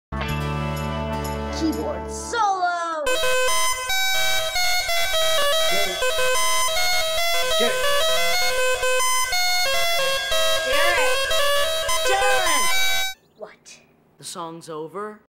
[Mobile] Keyboard Solo
Scholastic blocked the other video on mobile devices, so I removed some of the footage and added some wacky zooming